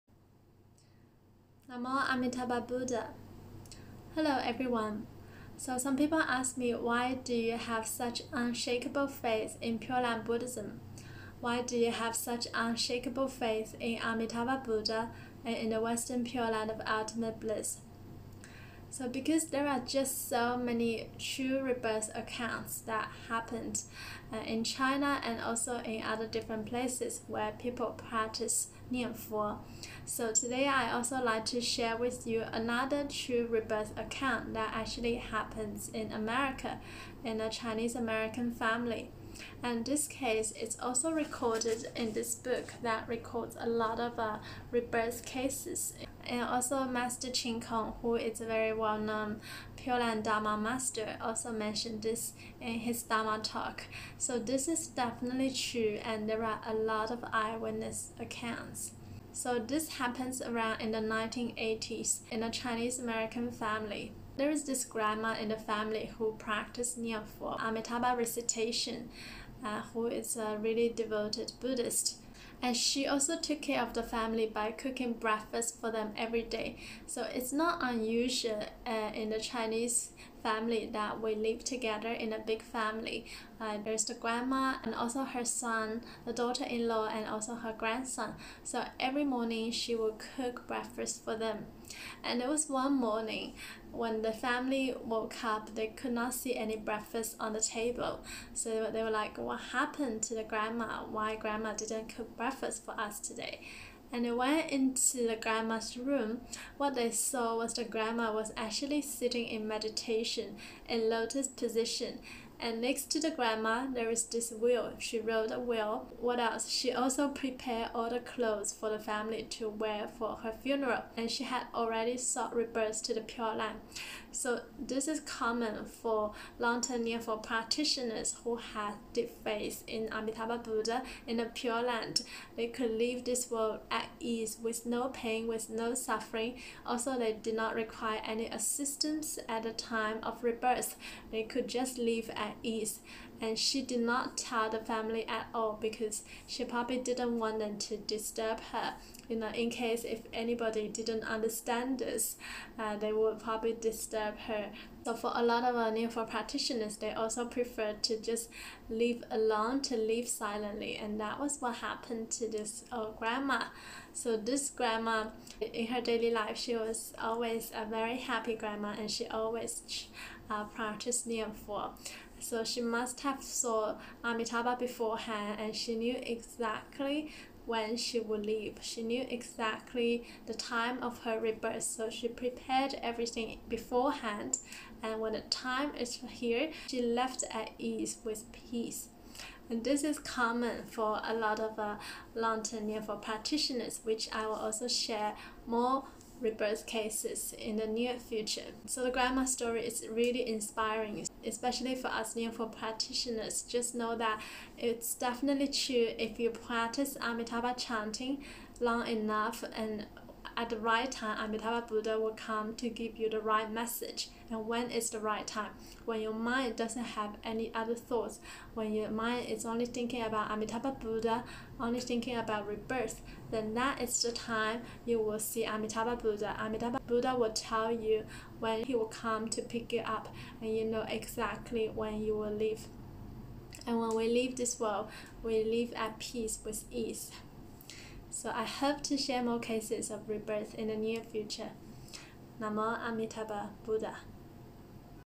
True Rebirth Account: A Grandma in the US Sought Rebirth Into the Pure Land
In this video I share how a grandma in the US was successfully reborn into the Pure Land by sitting in meditation. There are so many successful rebirth accounts which I will share more in the near future.

🙏 Namo Amitabha Buddha 🙏
🙏 Namo Amituofo 🙏